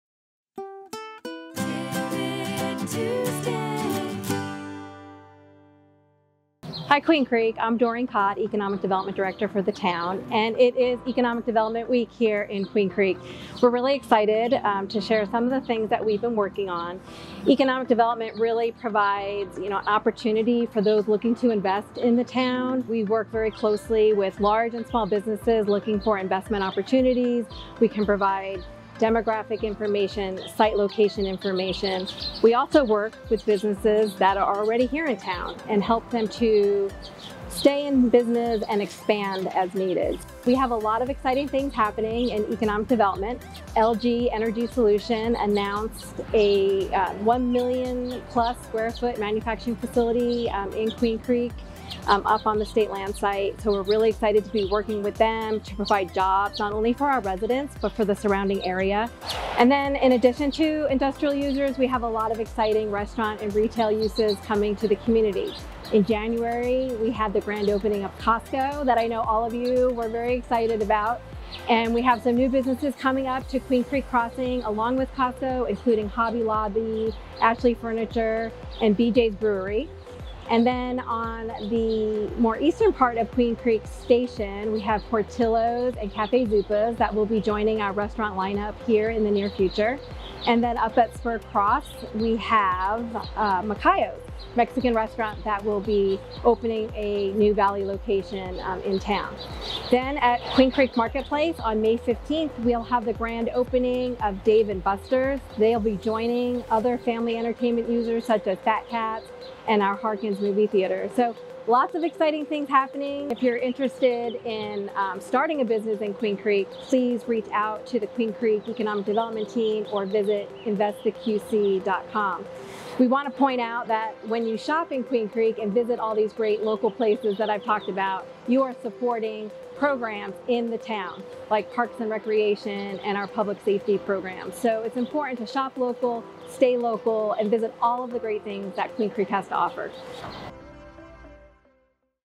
TidBit Tuesday - Economic Development Week
It's Economic Development Week! 🛍️ 🍽️

For this week's TidBit Tuesday, Economic Development Director Doreen Cott is here to provide some exciting updates!

To learn more about economic development in Queen Creek, please visit InvestTheQC.com